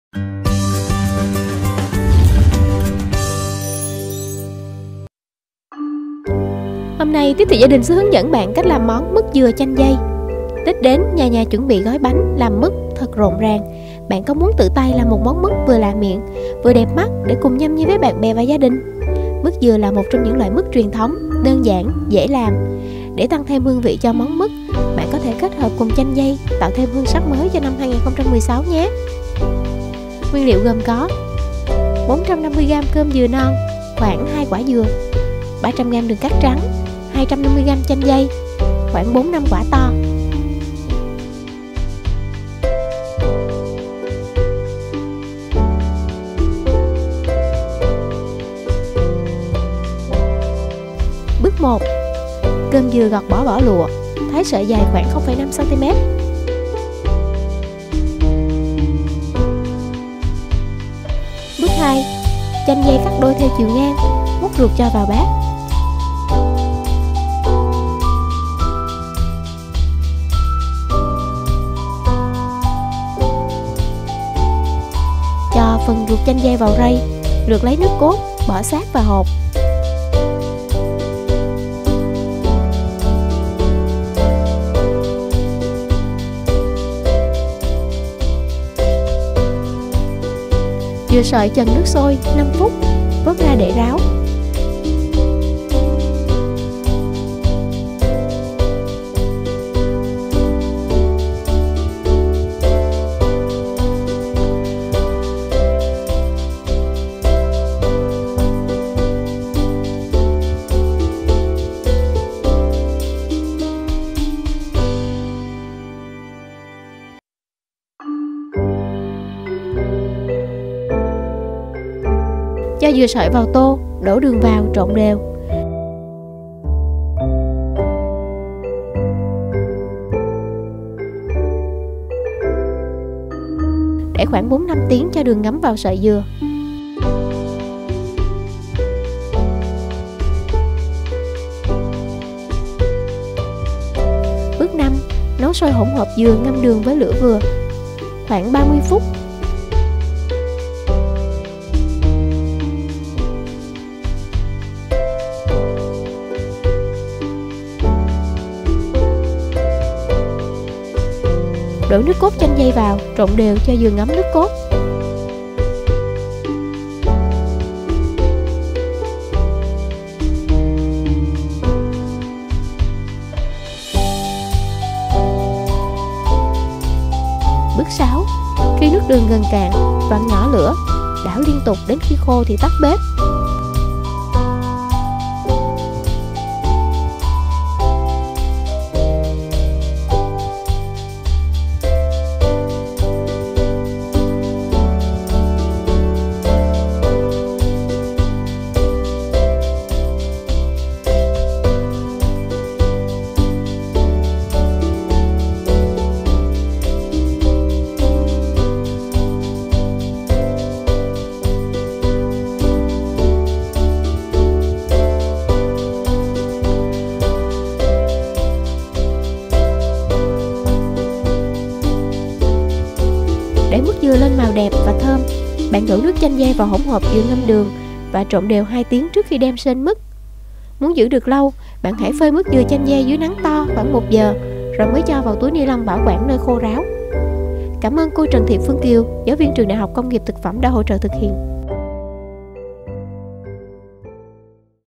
Hướng dẫn làm món Mứt dừa chanh dây - TTGĐ 01/2016
Tết năm nay gia đình bạn sẽ được thưởng thức một món mứt dừa với mới đó là mứt dừa chanh dây. Thướng thức món mứt này cùng với ly trà nóng trong tiết trời hơi se lạnh sẽ thật tuyệt với đấy bạn!! Để biết công thức và cách làm bạn hãy theo dõi video sau đây cùng TTGĐ nhé:)

Bạn có thể tham khảo cách chế biến các món ăn này theo hướng dẫn của TT&GĐ phiên bản iPad hoặc trên TTGĐ phiên bản in số 01/2016, ra ngày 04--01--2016.

*** Bạn có thể xem thêm nhiều món ngon khác tại tại

    Category
        Howto & Style
    License